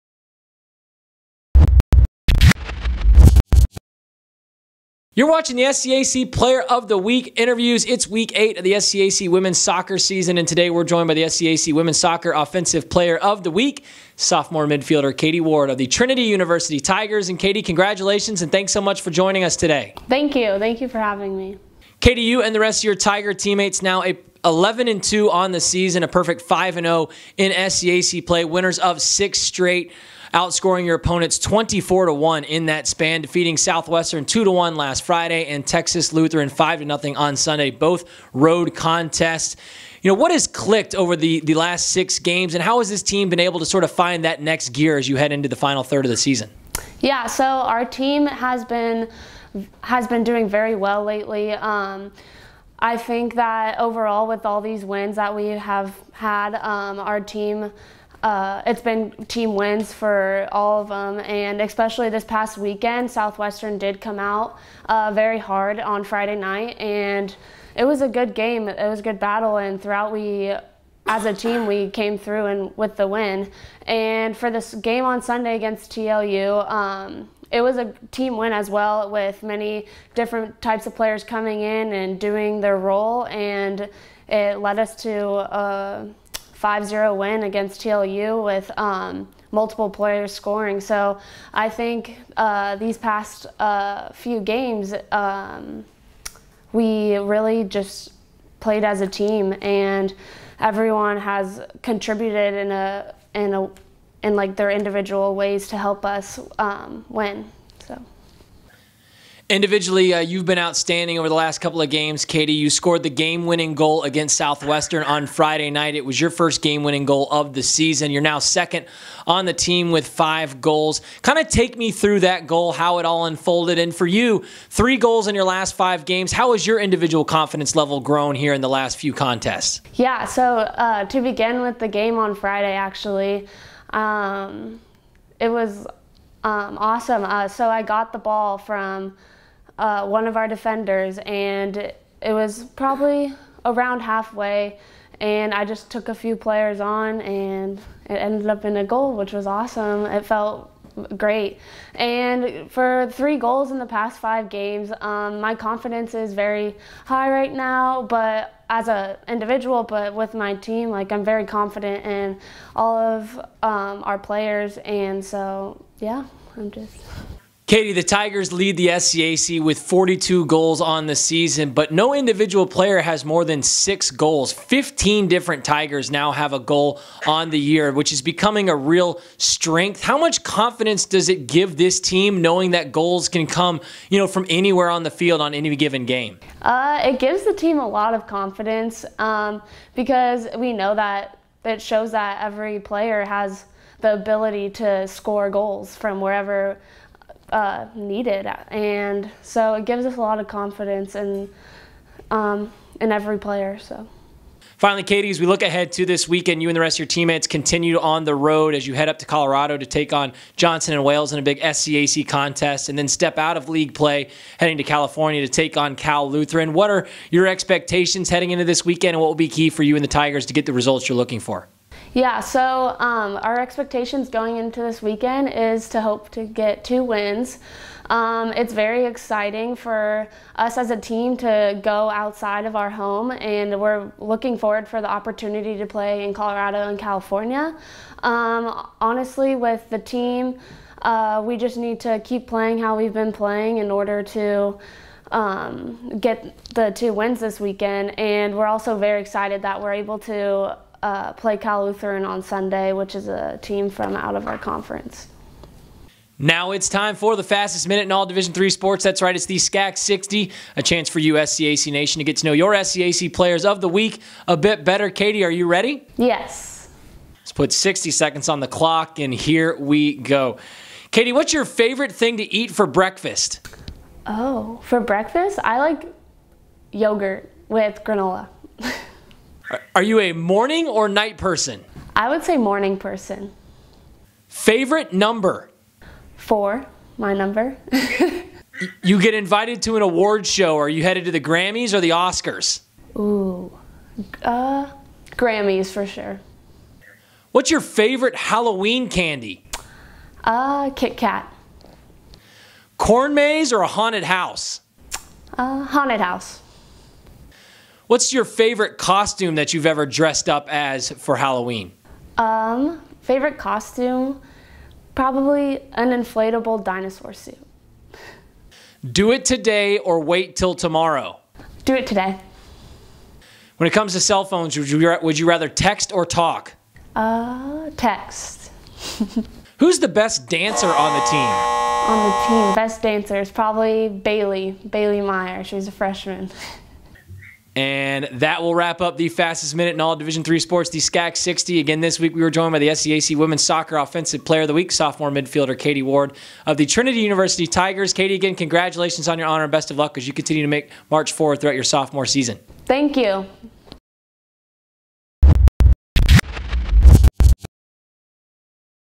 Kaity Ward, Trinity University, Offensive Player of the Week (Week 8)
Kaity Ward led Trinity’s 18th-ranked women’s soccer team to road wins over Southwestern and Texas Lutheran in SCAC play last weekend, keeping the Tigers in first place in the conference standings. Ward scored the eventual game-winning goal in Friday’s 2-1 win over Southwestern. She narrowly missed a goal on Sunday against Texas Lutheran, hitting the cross bar on her only shot of the afternoon. Ward is second on the team with five goals this season and Friday’s game-winner was her first of the season.